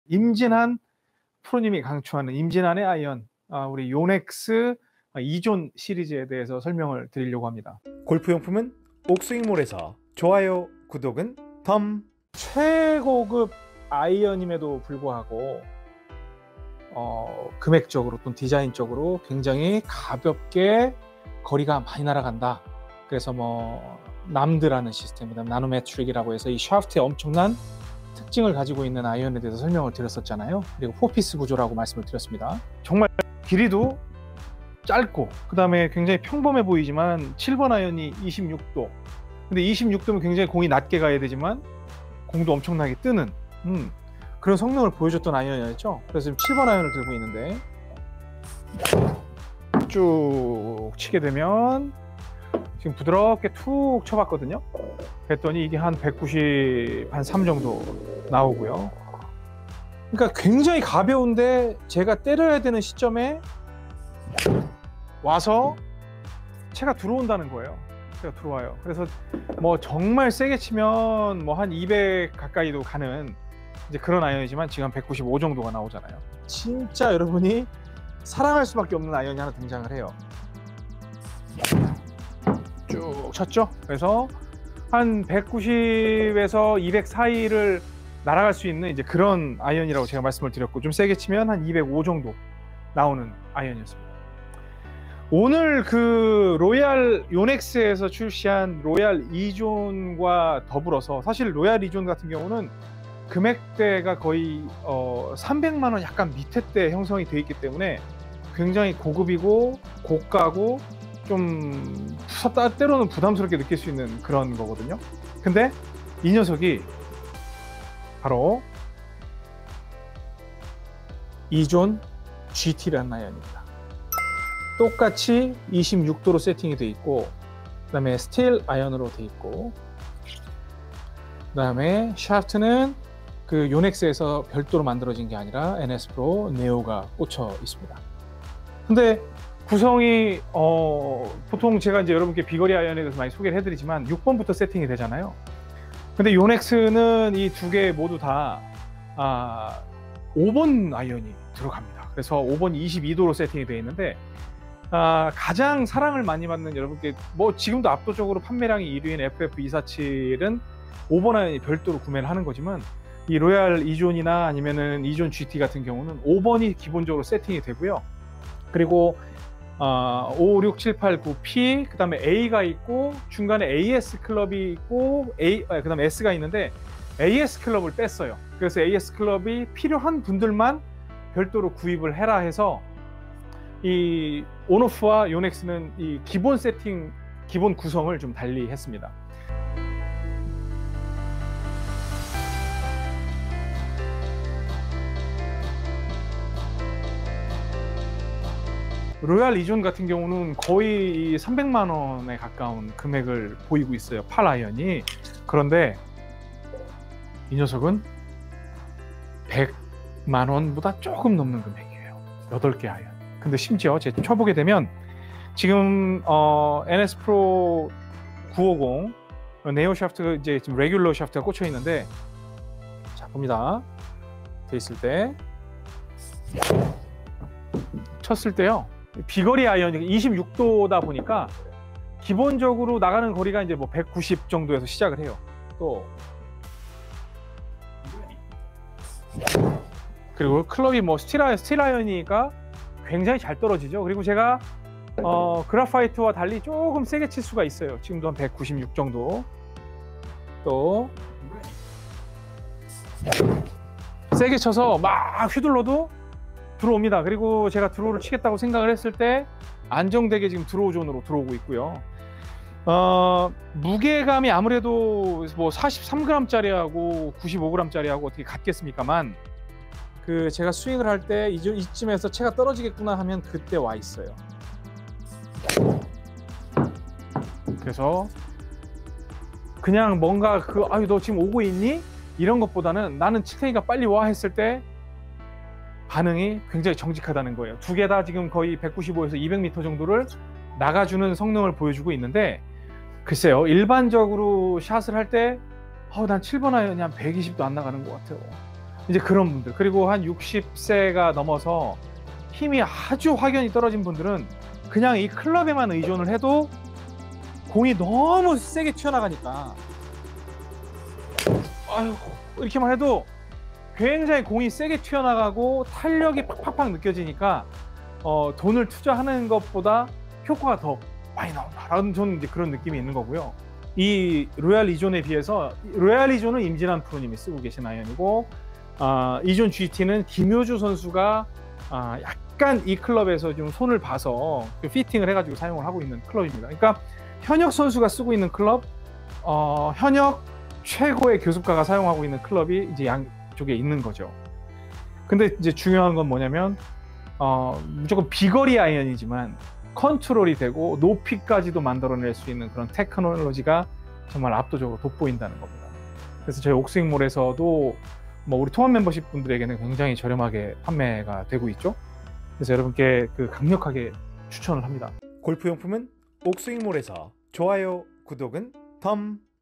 임진한, 김효주 프로의 극강의 비거리 아이언! 요넥스 로얄이존(시니어) & 이존지티(안시니어)
로얄이존 구매링크 (남성용)